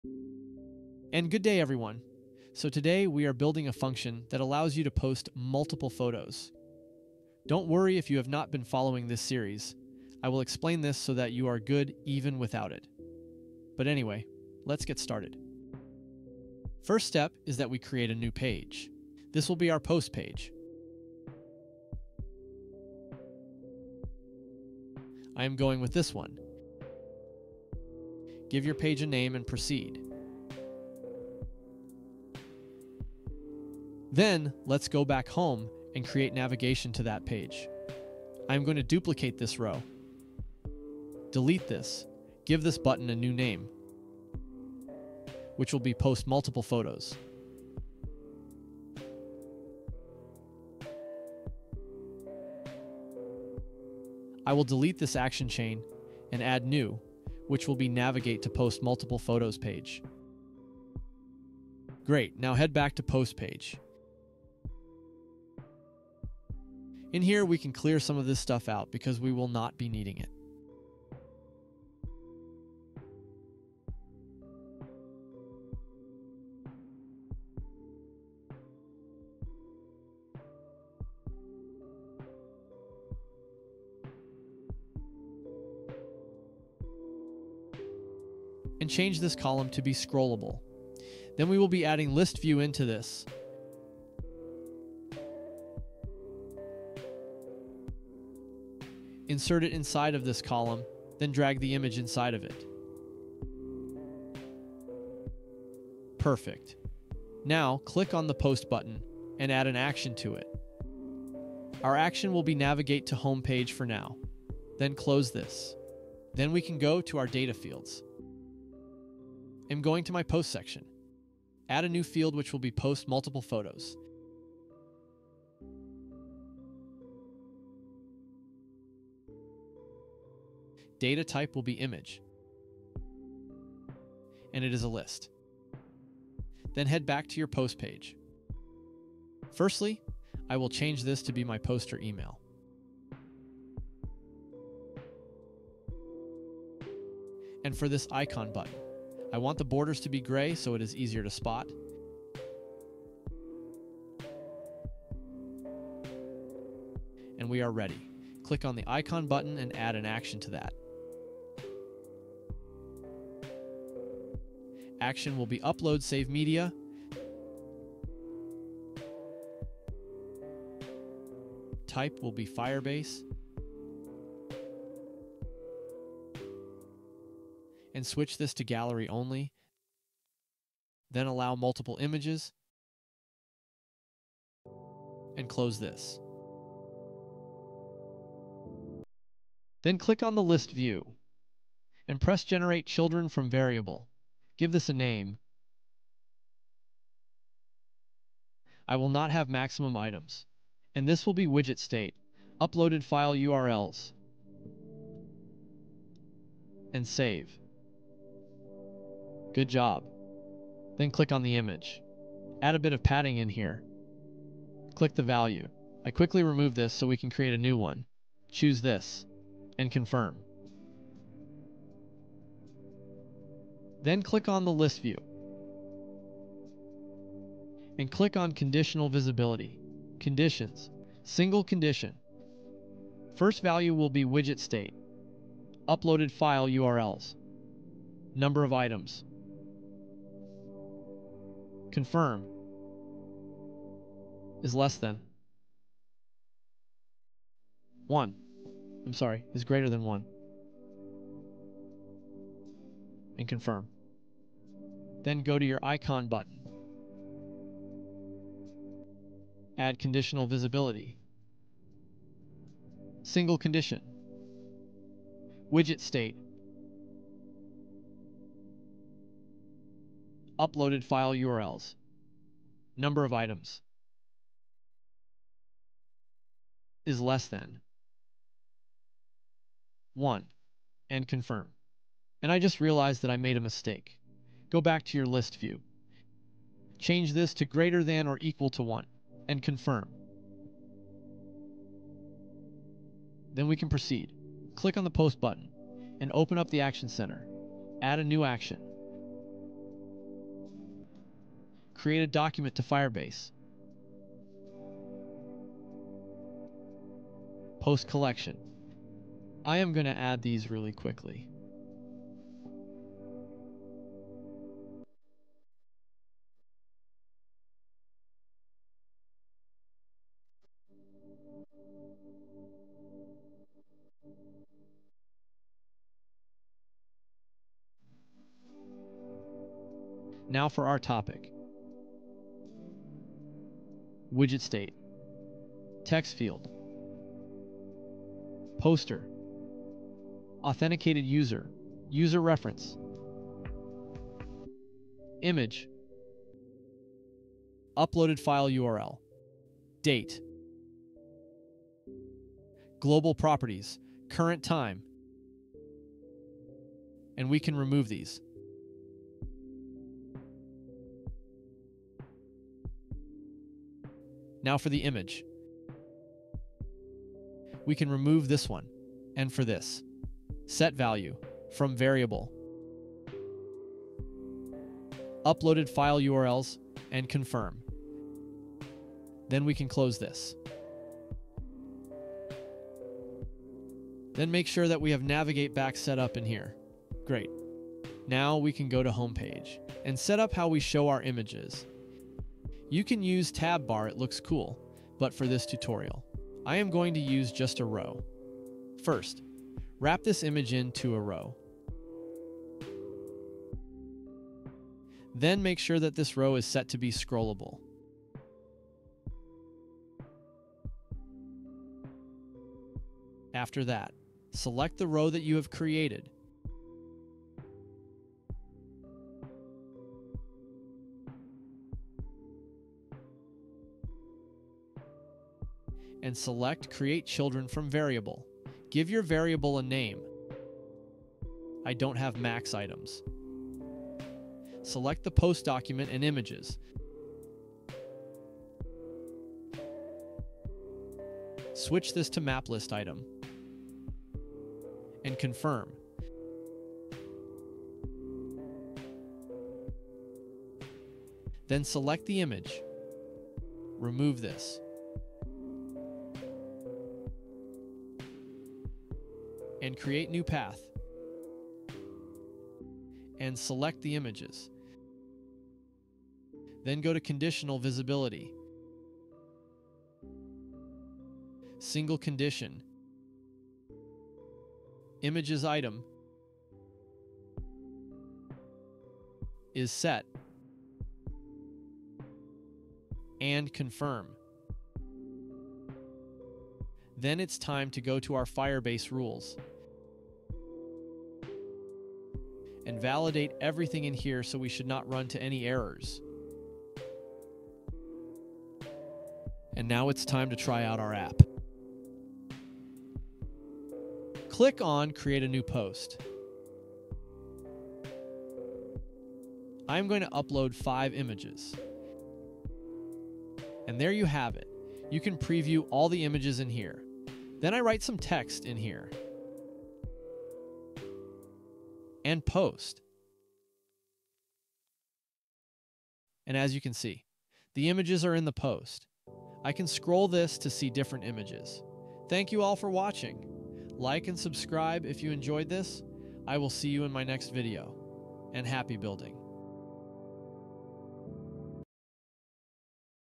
FlutterFlow - Upload Multiple Photos
Uploading and Displaying Multiple Photos in Your Social Media App | FlutterFlow Series Episode 5

This tutorial will cover how to enable users to upload multiple images, manage them, and display them effectively in your app’s feed or gallery. Follow along to learn how to enhance your app’s multimedia capabilities and provide a seamless photo-sharing experience for your users.